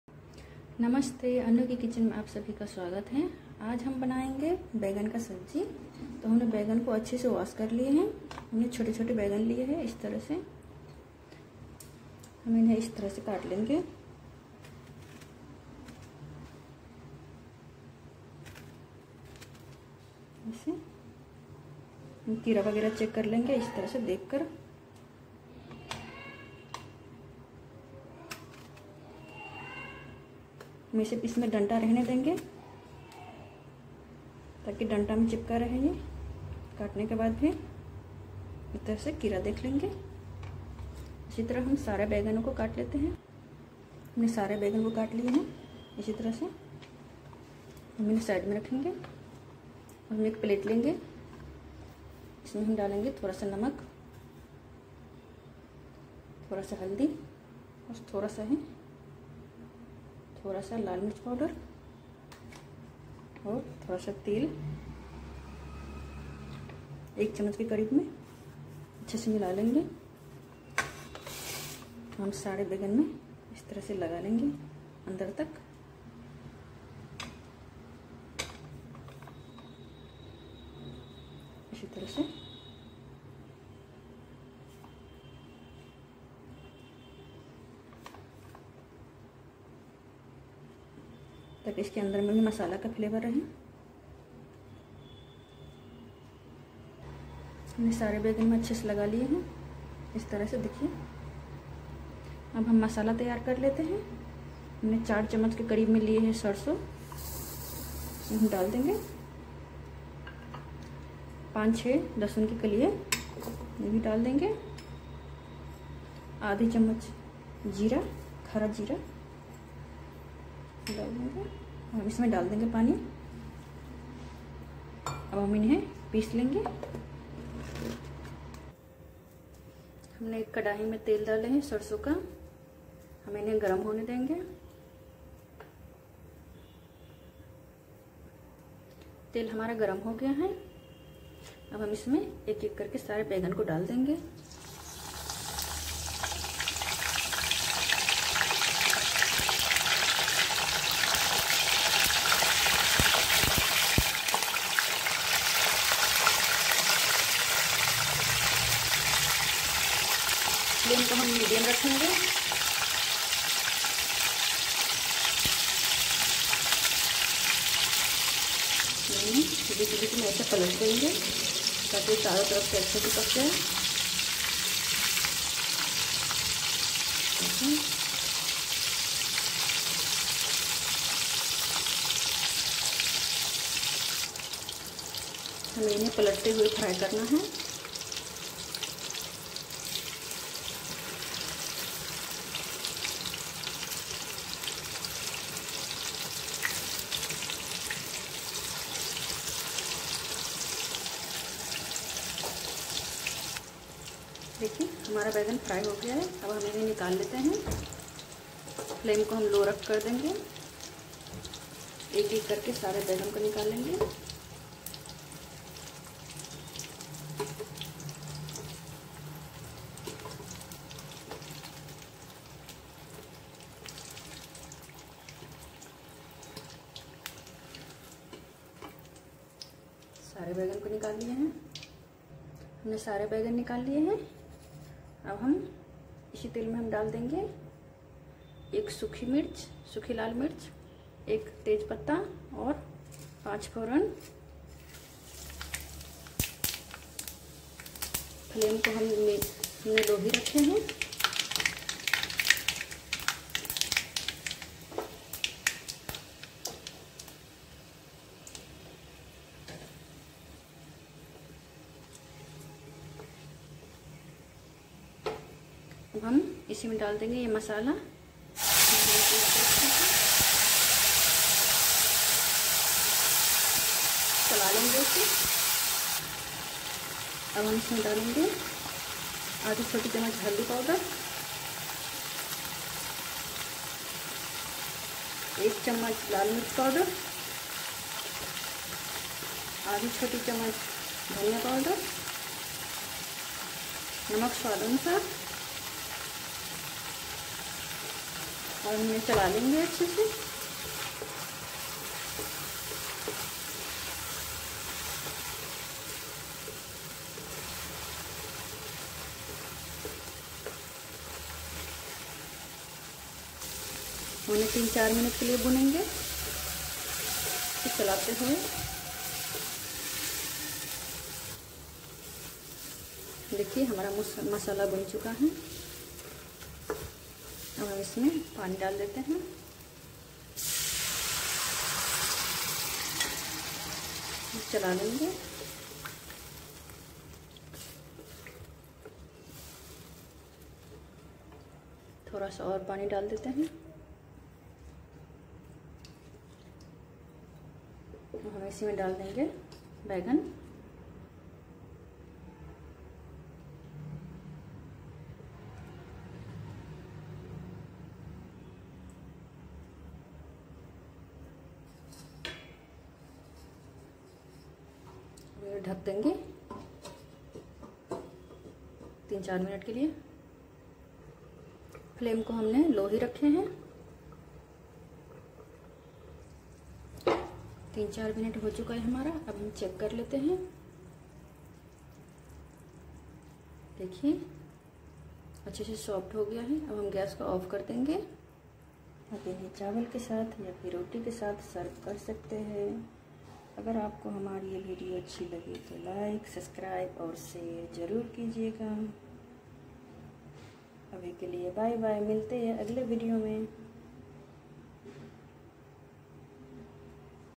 बैंगन का मसालादार सब्जी ऐसे बनाएंगे तो सब अंगलिया चाट कर खाएंगे | Brinjal Masala recipe
Ingredients

250 gm Baigan ( Brinjal)
100 gm Mustard oil
4tsp mustard
1tsp Jeera
1tsp Garlic
1tsp Red Chili powder
1/2tsp Turmeric powder
1/2tsp coriander powder
Salt to taste
1 Tej leaf
1/2tsp Panchforan
1 piece Red chili
400 gm water